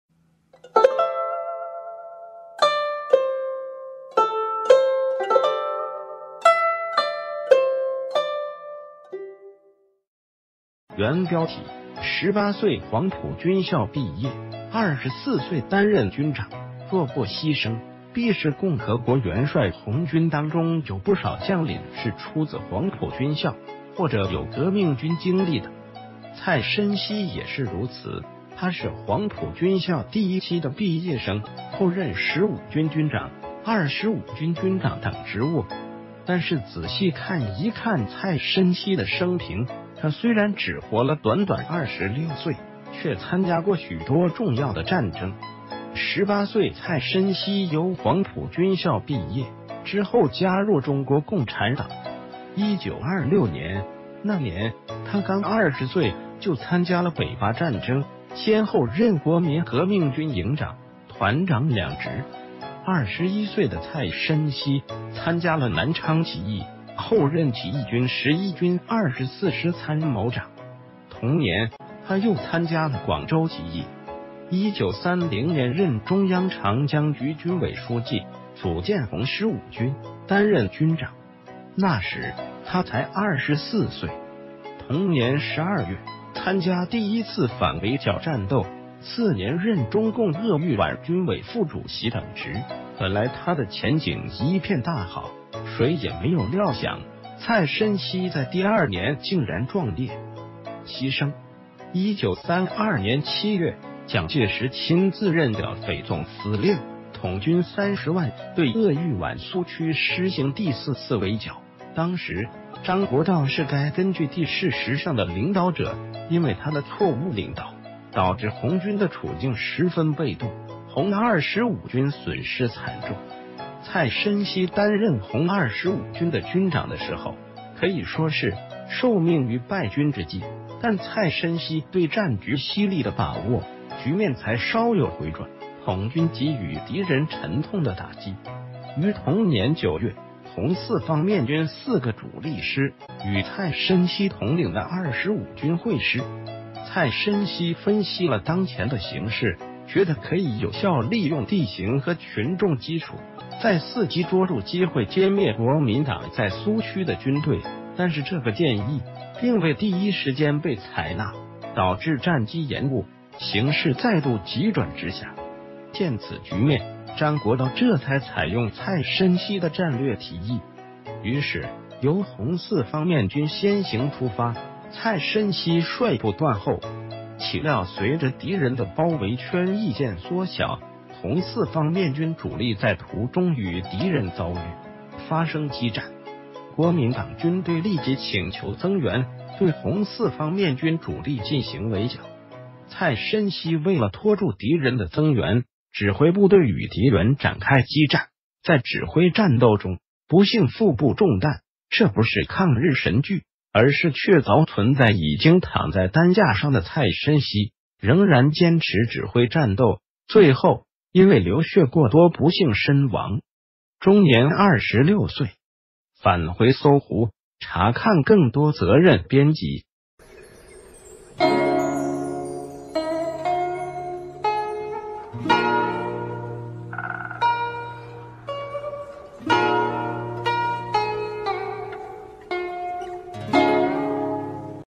18歲黃埔軍校畢業，24歲擔任軍長，若不犧牲必是共和國元帥_蔡申熙
原標題：18歲黃埔軍...